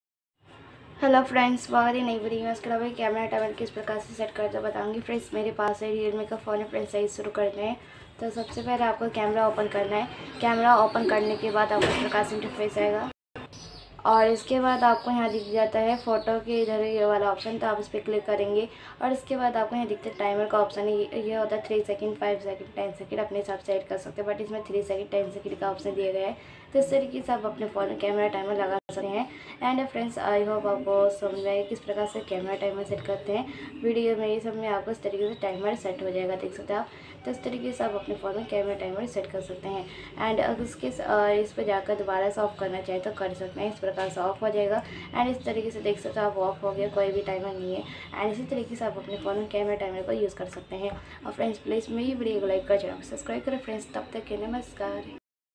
Realme narzo 50 camera time setting | How to set camera time | camera timer use kaise kare
how to use camera Timer in Realme narzo 50
camera ka time kaise set karen
camera ka time kaise hataye

Google search
Techno Puja Technopuja@Technopuja